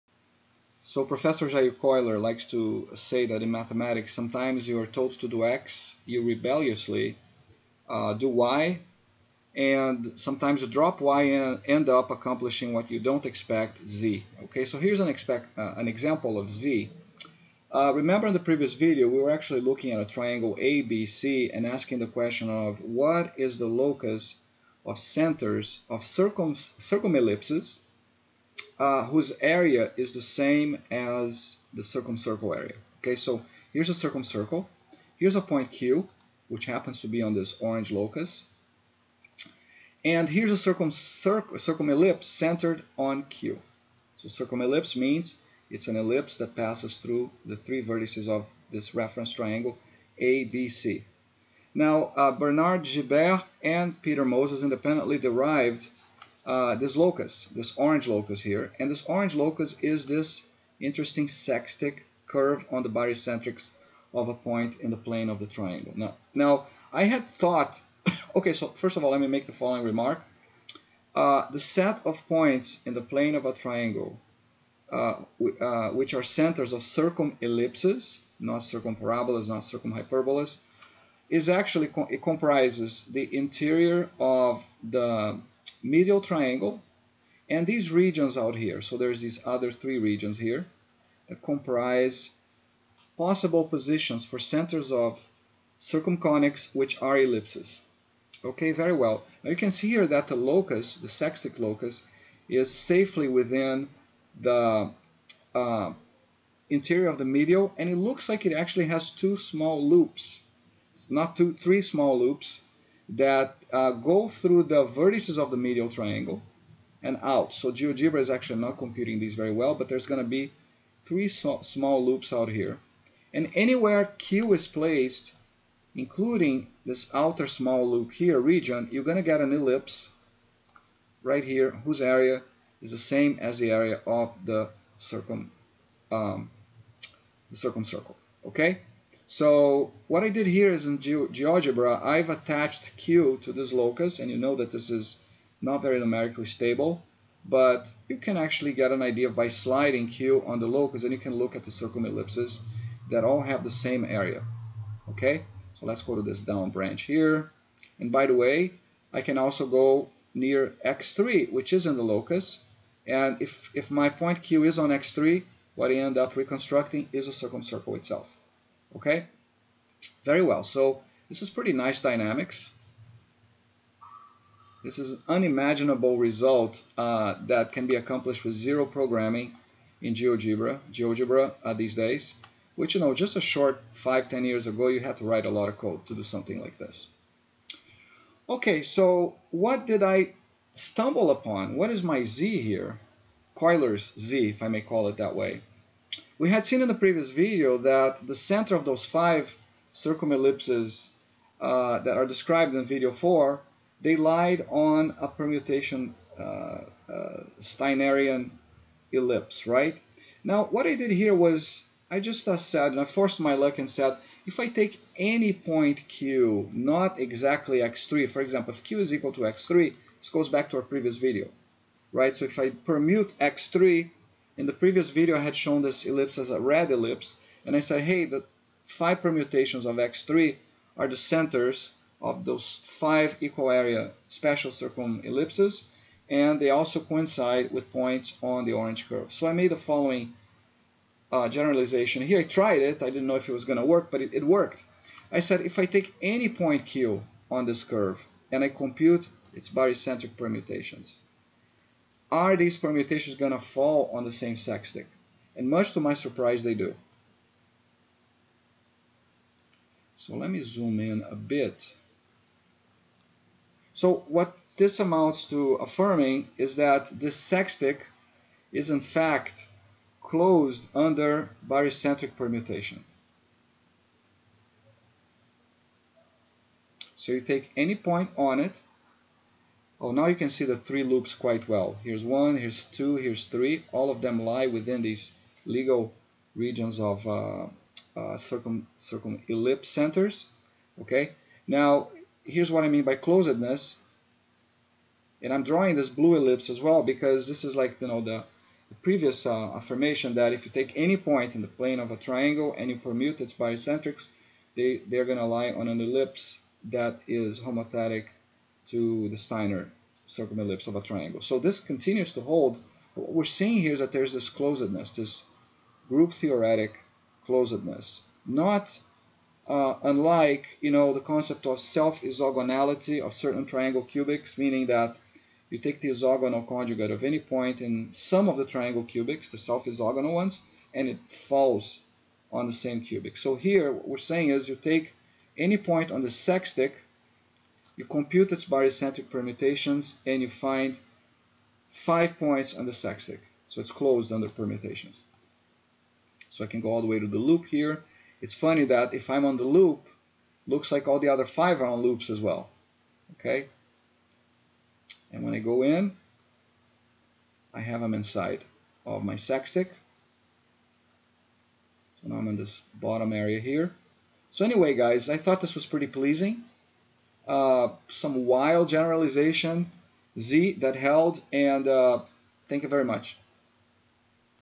Permutation Ellipses V: The Self-Permuting Sextic
In Mathematics, you are sometimes told to do X, you rebelliously try Y, and end up accomplishing Z. --Jair Koiller.

In this video we show that the locus of centers of circumellipses whose are is the same as the circumcircle's is closed under permutation of barycentric coordinates. I am calling this "self-permuting", after Gibert's "self-isogonal" cubics.